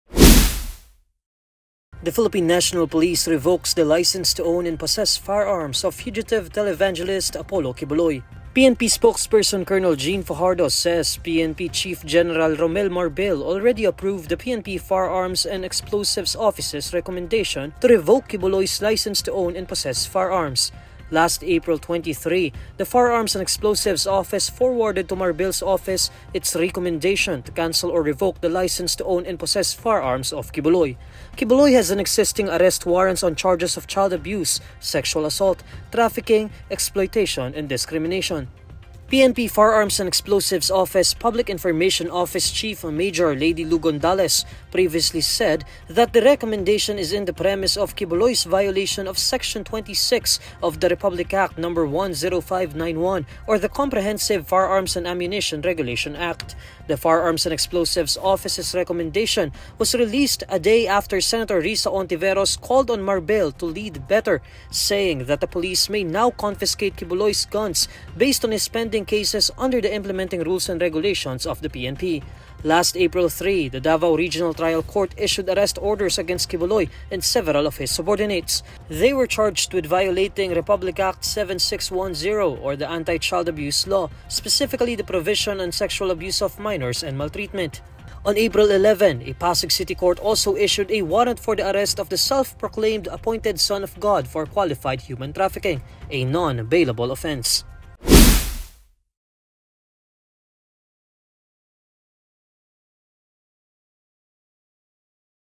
PNP revokes Quiboloy’s firearm licenses | INQToday
The Philippine National Police revokes the license to own and possess firearms of fugitive televangelist Apollo Quiboloy.